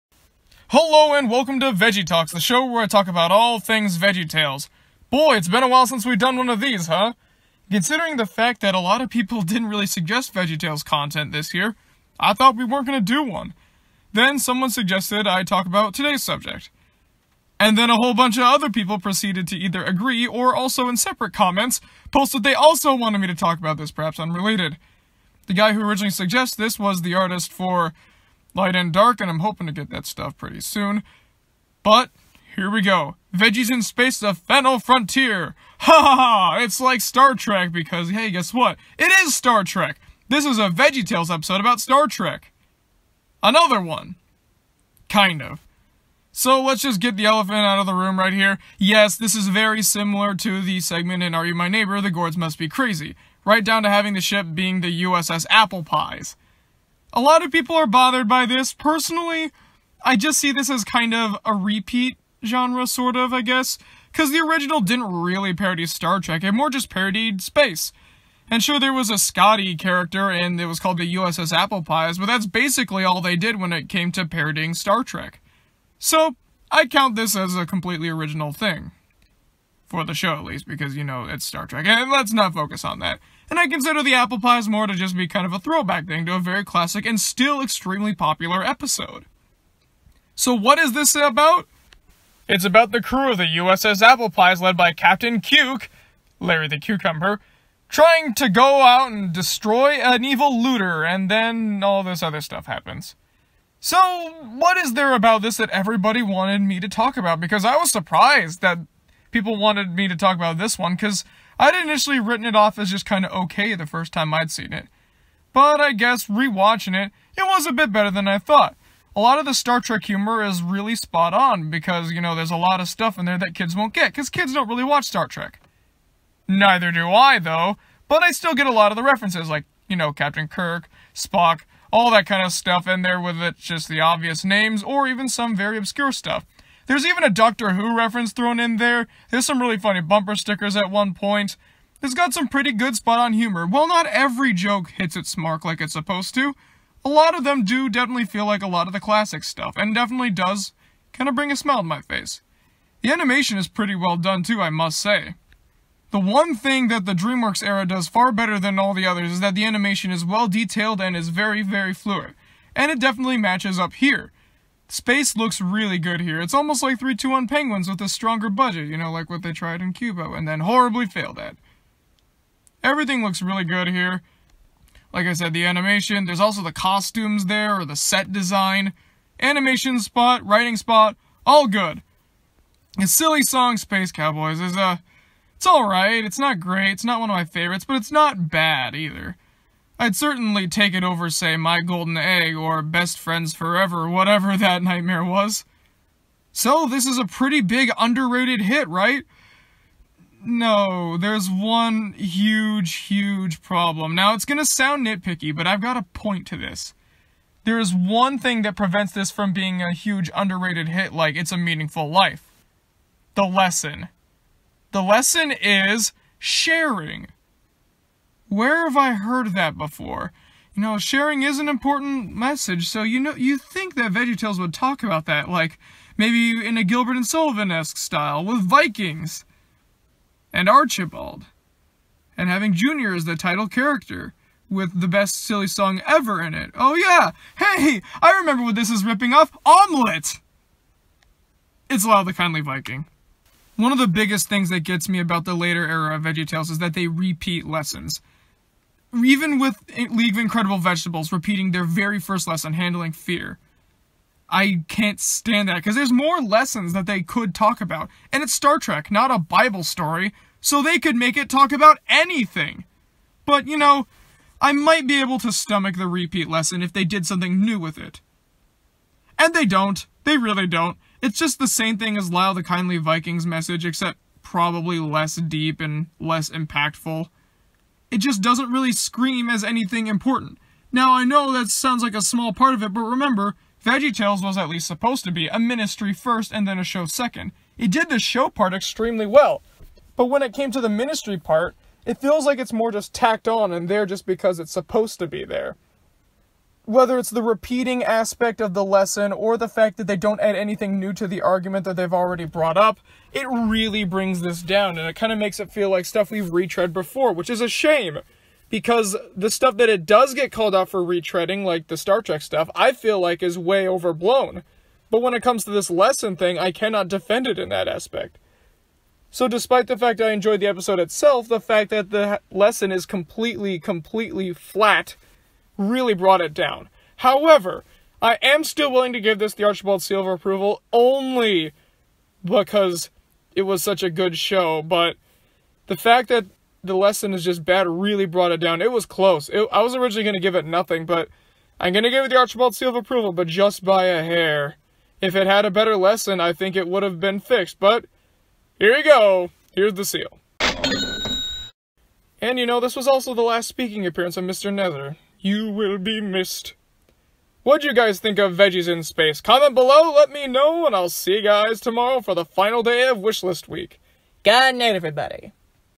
VeggieTalks: Veggies In Space-The Fennel Frontier
I talk about this very divided episode, and what works about it, and what doesn't.